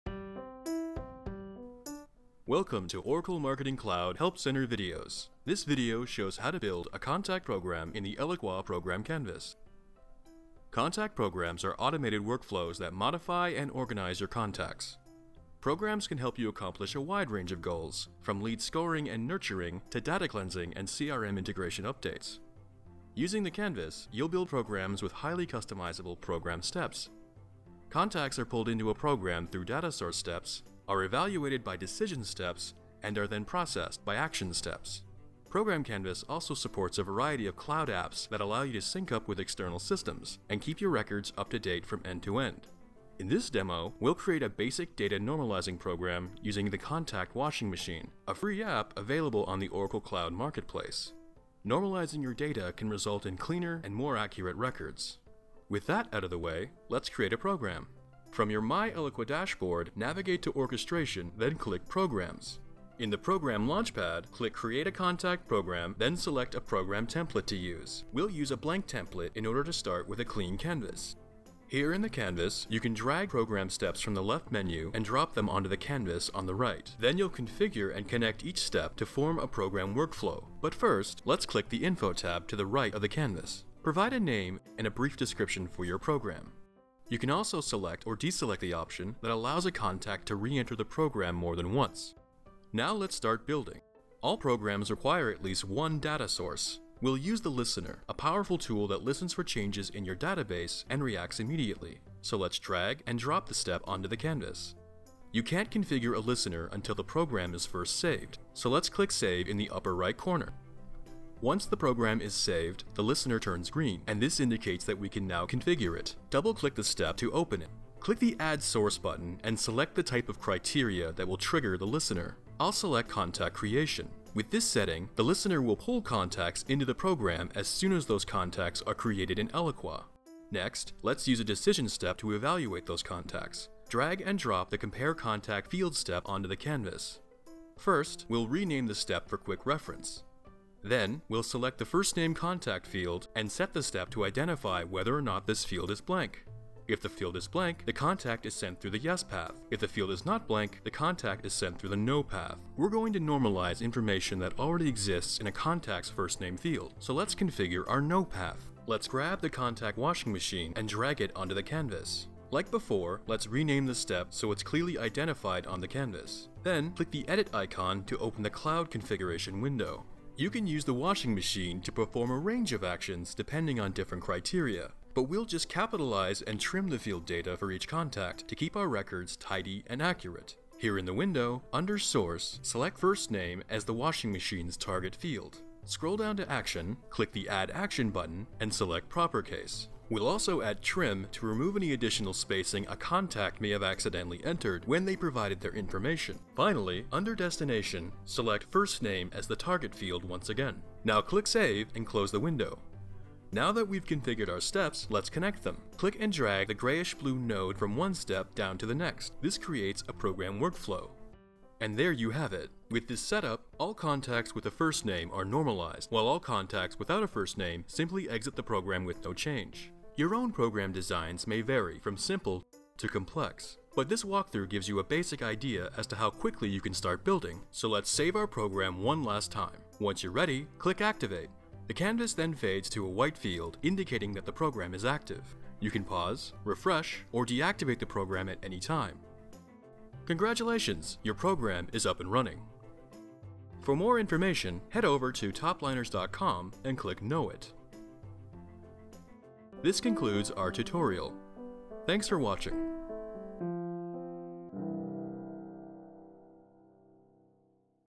Oracle Eloqua - Creating a Contact Program
Learn how to quickly build contact programs in Eloqua's program canvas.